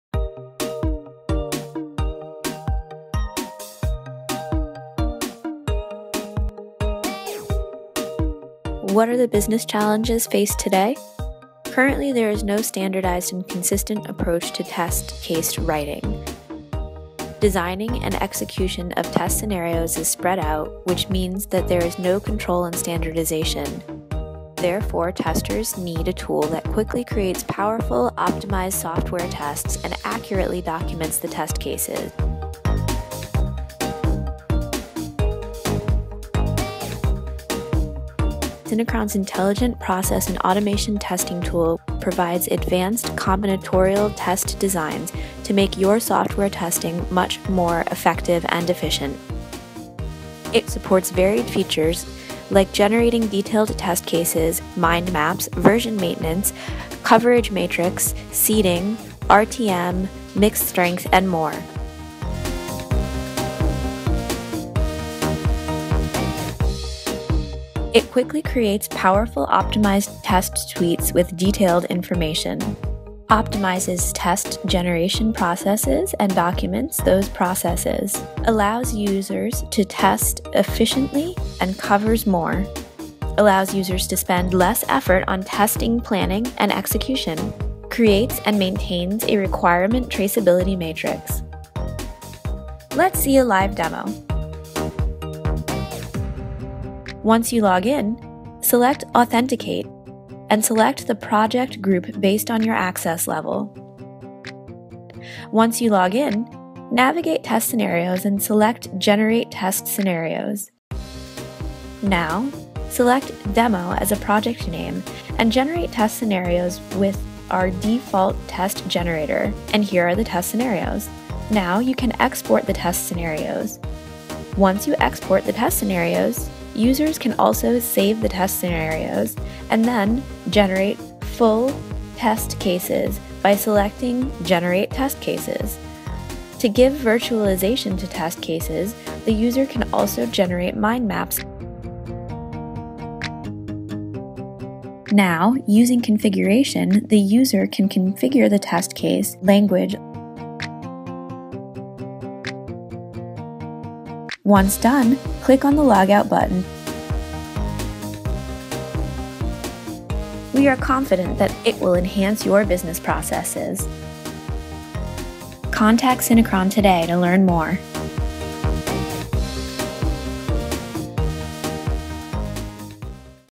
Syne Intelligent Process Automation in Testing Demo
Innovation in Quality Assurance is a key focus for financial institutions and Synechron is using automated testing to help financial institutions embrace that change.